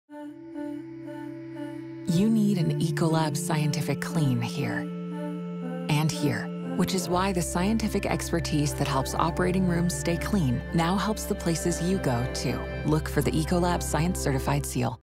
Ecolab Science Certified™: Restaurant
It’s not just any clean, it’s scientific clean. Designed by the experts who help keep hospitals clean, the Ecolab Science Certified program brings scientific cleaning expertise to the places you eat, stay, shop and play. We combine our hospital disinfectants and food-contact sanitizers proven effective against the COVID-19 virus and other germs with comprehensive training, procedures and audits to help deliver a higher level of cleanliness. So you can feel more confident visiting the everyday places that have earned the seal.